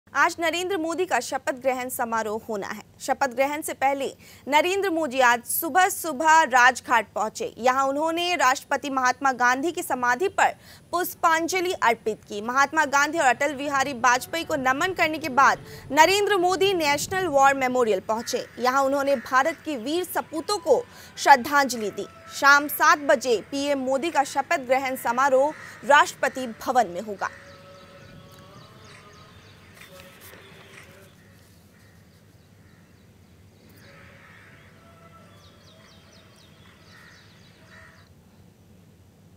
शपथ से पहले महान सपूतों को नमन!...
शपथ से पहले महान सपूतों को नमन!...राजघाट और सदैव अटल के बाद वॉर मेमोरियल पहुंचे मोदी…शपथ ग्रहण से पहले PM मोदी ने बापू-अटल को किया याद…शहीदों को दी श्रद्धांजलि…आज तीसरी बार प्रधानमंत्री बनेंगे मोदी…